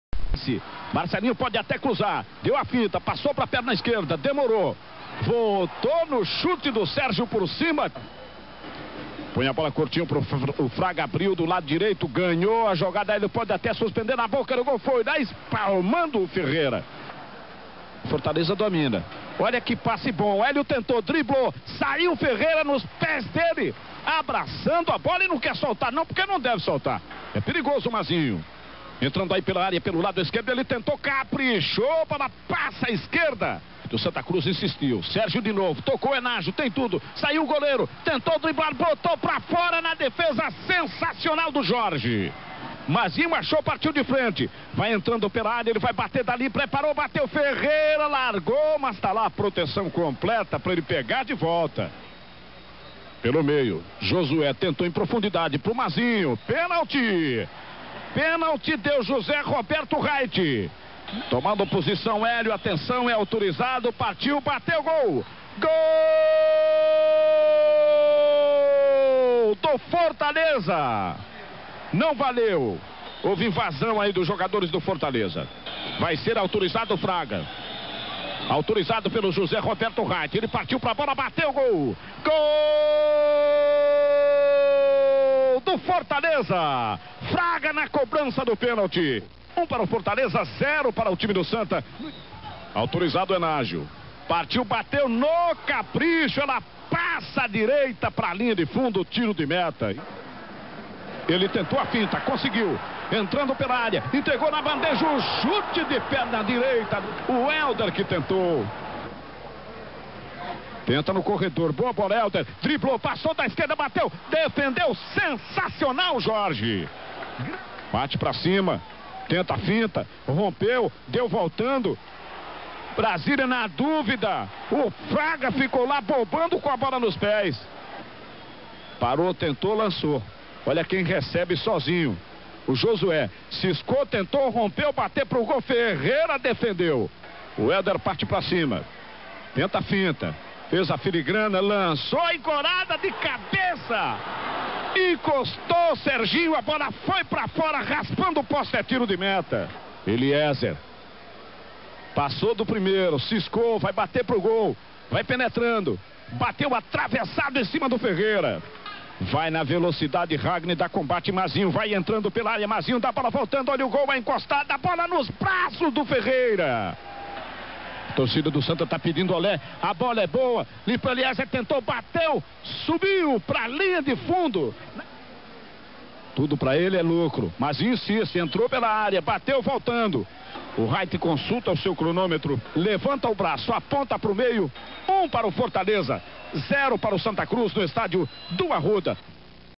Santa Cruz 0 x 1 Fortaleza (Série B 1992)
Campeonato Brasileiro 1992 - Estádio do Arruda - Gol do Fortaleza: Airton Fraga